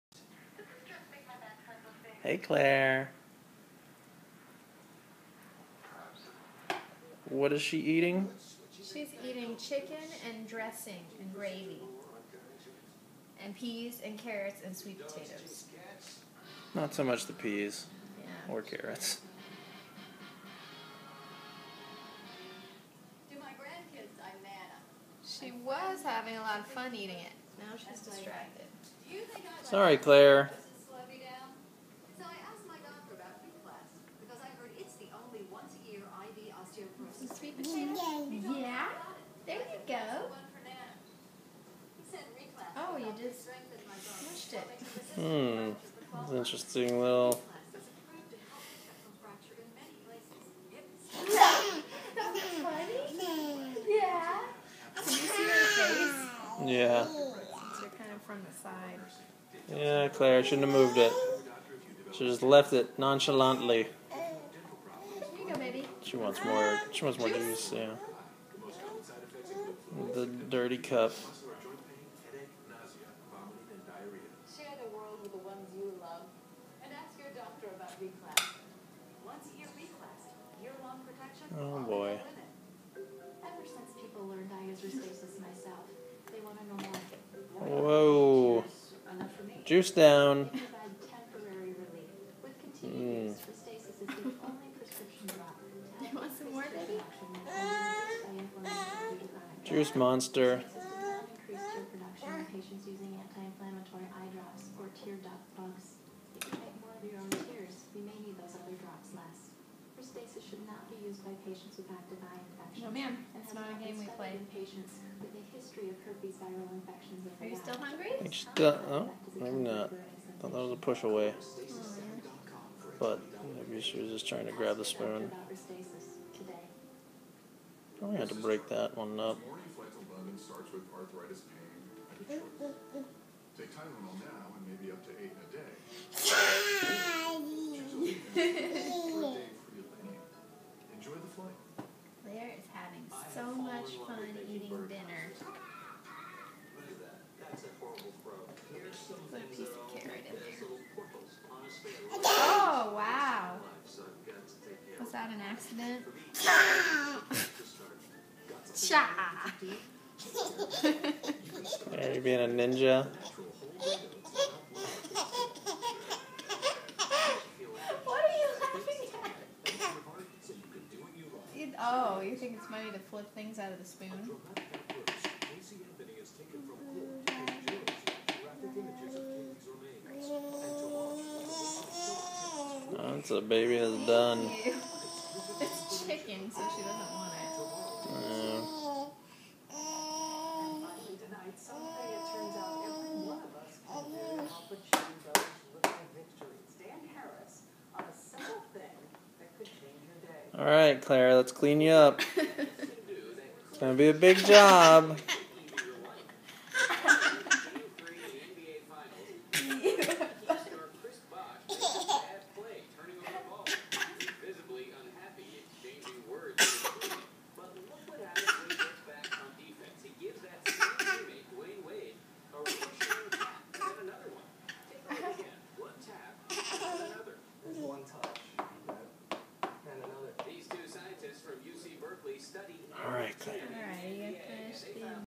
Claire enjoying her chicken and stuffing
And peas carrots sweet potatoes